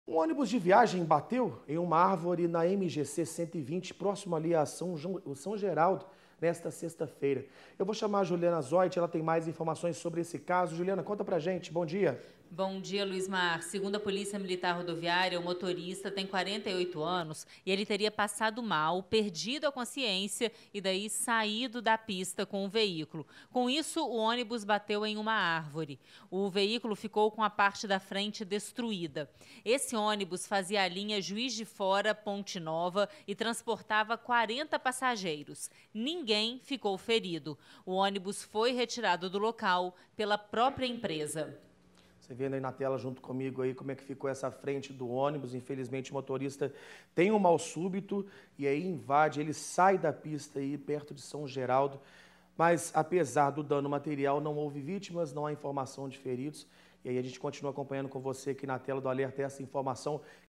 Motorista passa mal e bate com ônibus em árvore
Um ônibus de viagem bateu em uma árvore na MGC-120, próximo a São Geraldo, nessa sexta-feira (27).